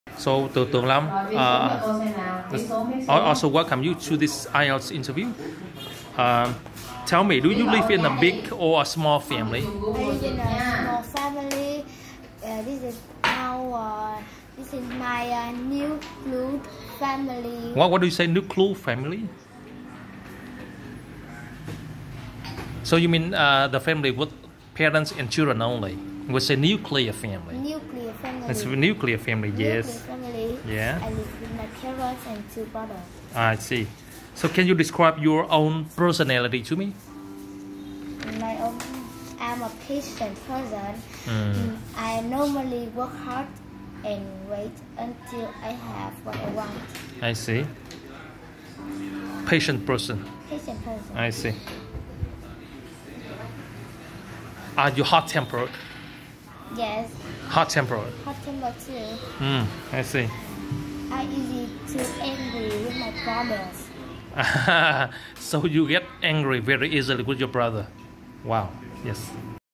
IELTS 4.5 - Tường Lam - "What kind of family are you living in?"
Nuclear family? Extended family (with many generations), DINK family (Dual Incomes, No kids)???